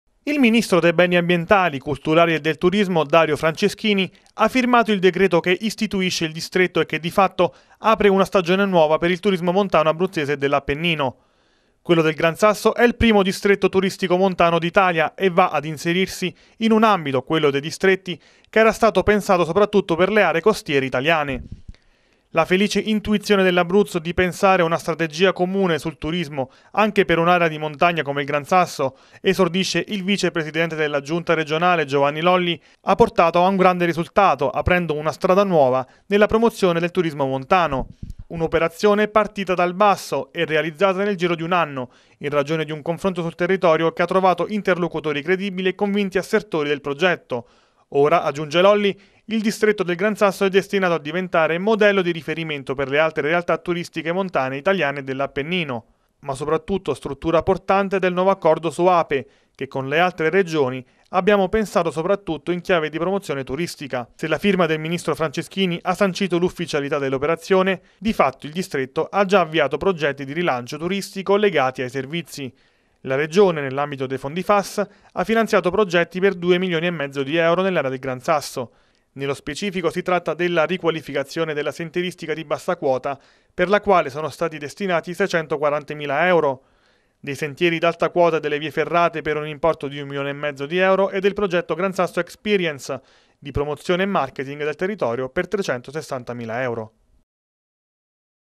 Nasce il distretto turistico del Gran Sasso
Quello del Gran Sasso è il primo distretto turistico montano d’Italia. L’ufficialità è arrivata con la firma del decreto da parte del ministro con delega al Turismo, Dario Franceschini. Previsti fondi per il ripristino della sentieristica, sentieri d’alta quota e marketing